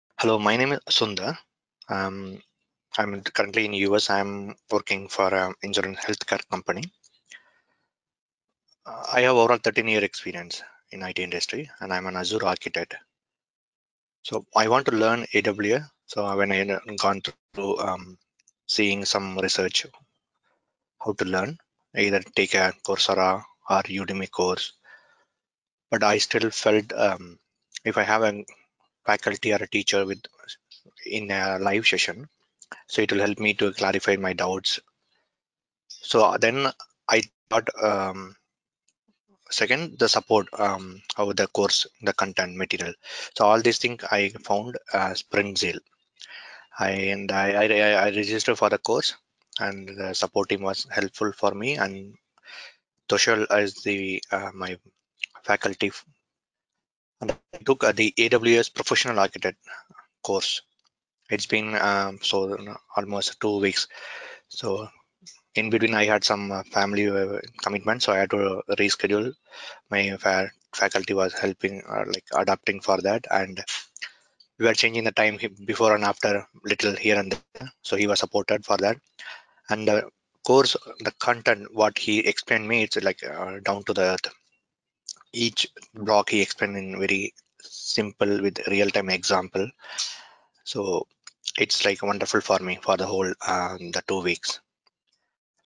AWS Solution Architect Certification | AWS Solutions Architect
Here is Our Participant Sundhar  Review About Our AWS Solution Architect Certification training From Sprintzeal